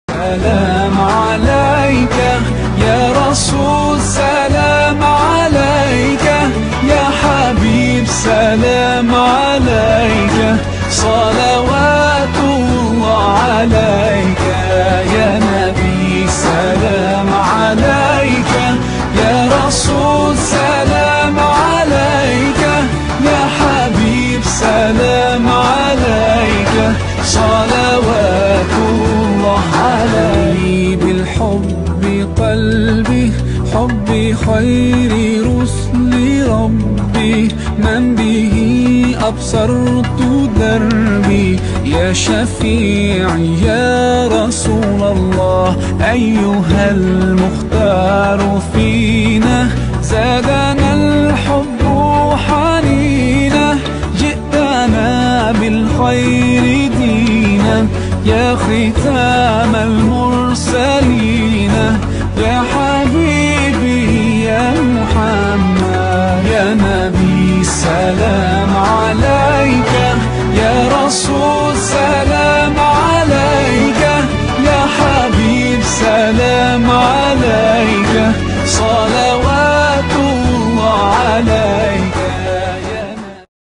Mountain of Uhud - the mountain that loved our last messenger of God!
Let history mark that day - the day when our last Prophet battled along with his faithful companions - The Battle of Uhud!